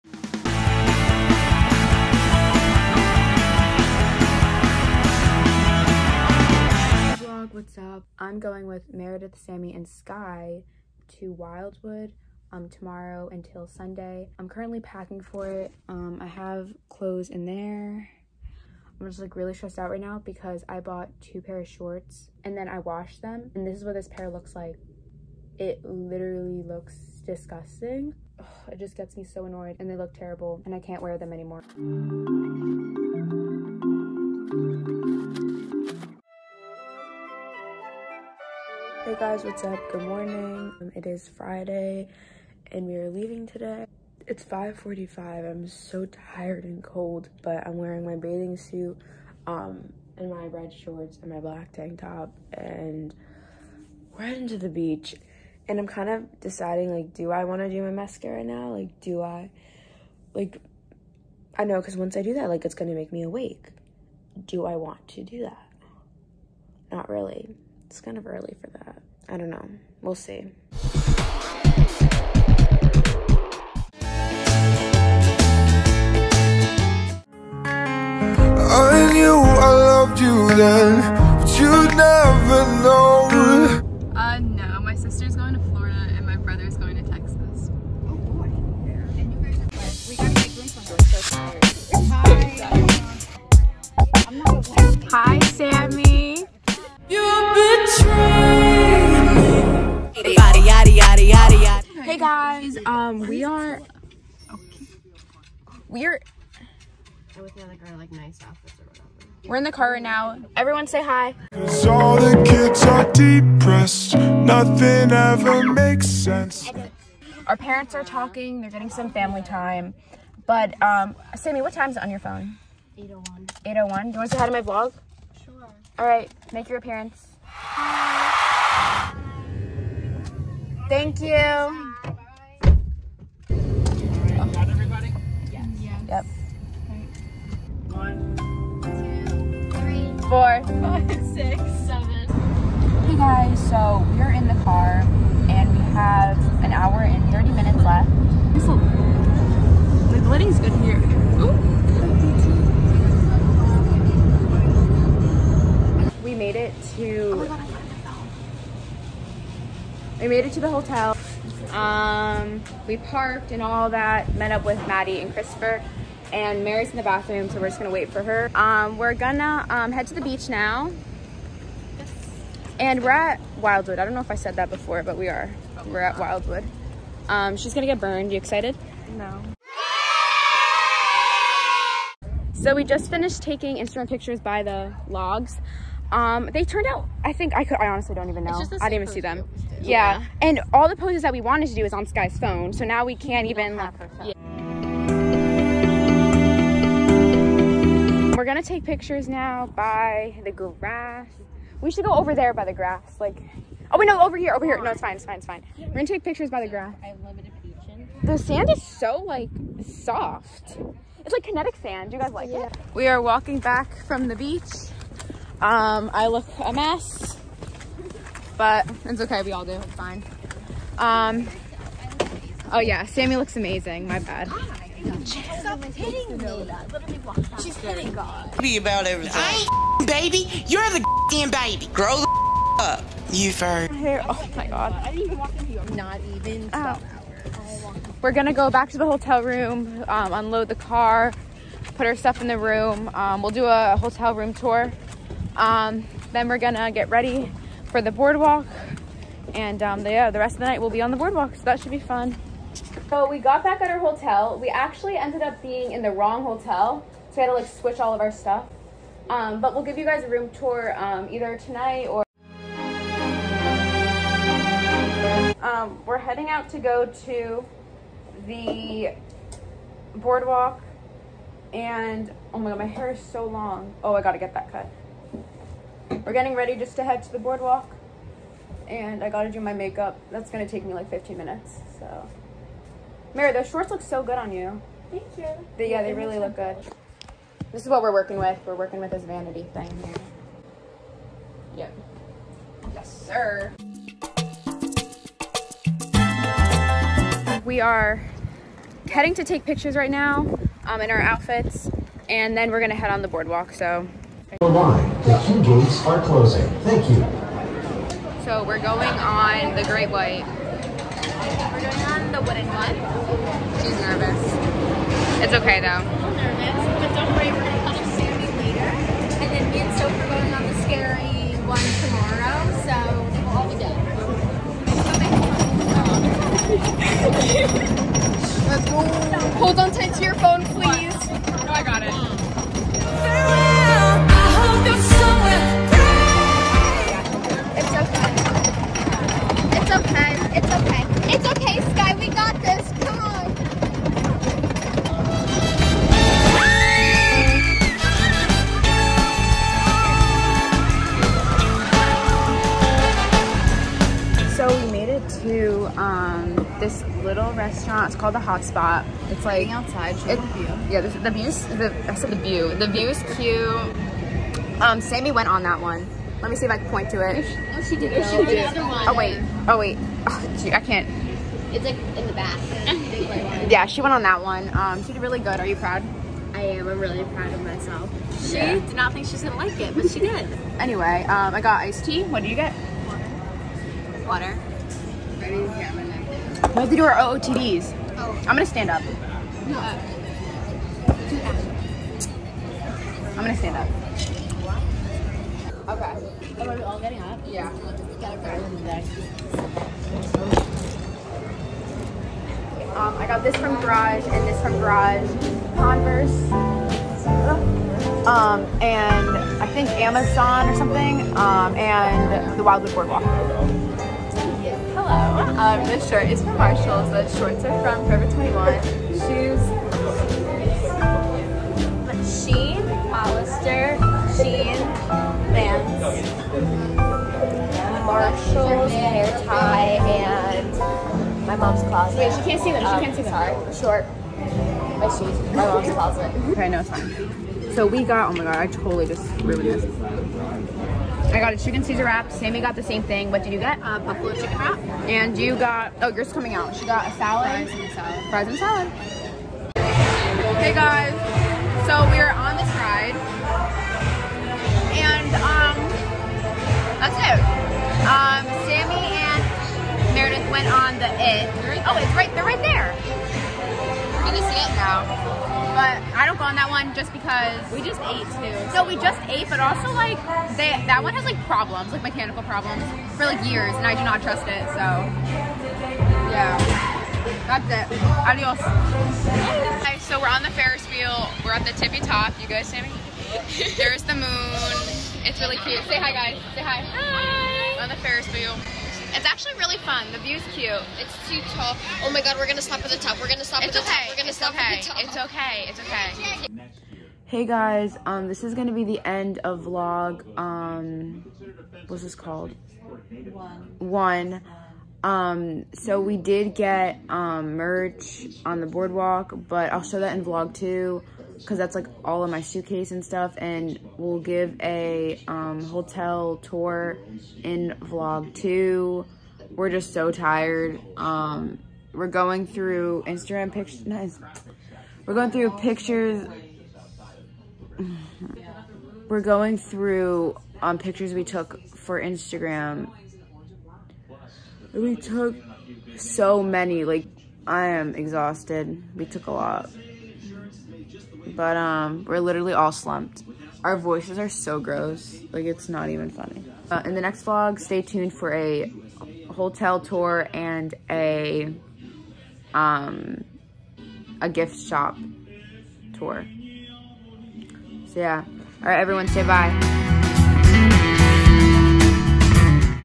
2021 GIRLS TRIP to wildwood nj / day 1
me and my friends went to wildwood for the week hope you enjoy vlog day one!! we gonna make it a traditionn